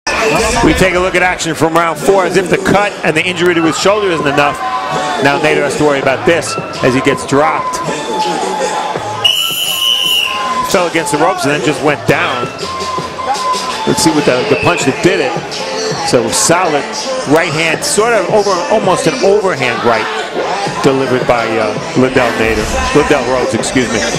Lydell Rhodes vs. John Nater - Round 4 KO
Highlights from the 4th round of Rhodes vs. Nater from the August 16 Showtime Extreme broadcast.